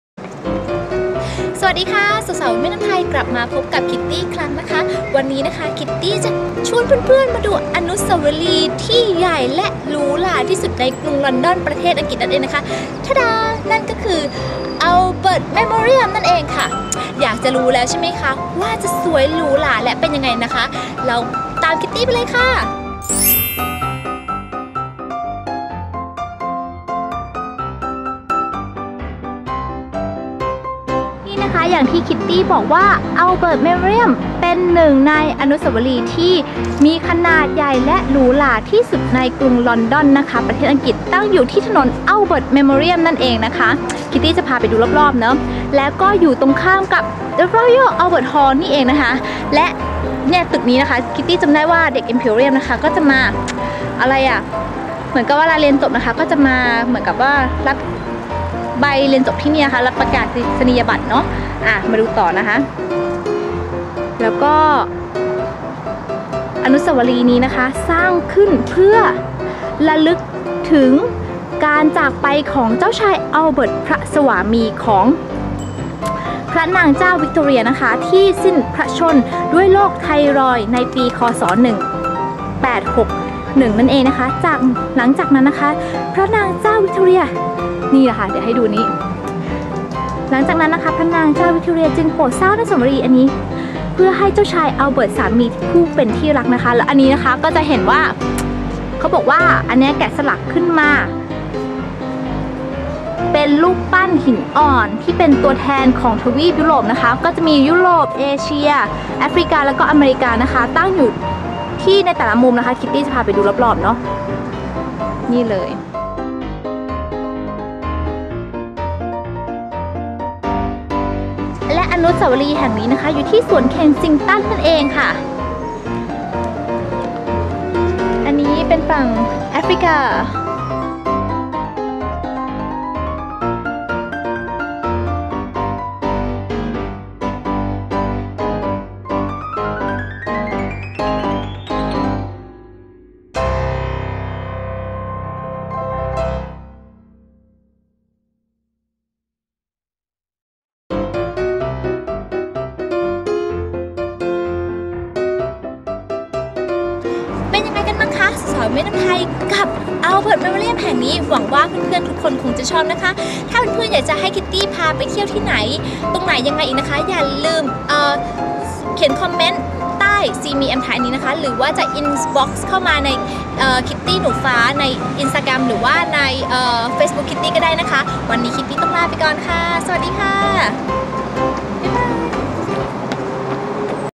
พาเที่ยวชม อนุสาวรีย์ Albert Memorial หนึ่งในอนุสาวรีย์ที่หรูหราที่สุด ในกรุงลอนดอน ประเทศอังกฤษ
สวัสดีค่าสาวๆ Women MThai ทุกคนนะคะ วันนี้คิตตี้พามาเที่ยวชมอนุสาวรีย์ Albert Memorial ซึ่งเป็นหนึ่งในอนุสาวรีย์ที่หรูหราที่สุดในกรุงลอนดอน ซึ่งสิ่งก่อสร้างนี้ ถูกสร้างขึ้นเพื่อเป็นเกียรติแก่เจ้าชาย Albert อนุสาวรีย์นี้อยู่ในสวน Kensington พระนางเจ้าวิคทอเรีย สร้างขึ้นเพื่อระลึกถึงพระสวามีอันเป็นที่รัก

อ่านบทความต่อเพิ่มเติมได้ที่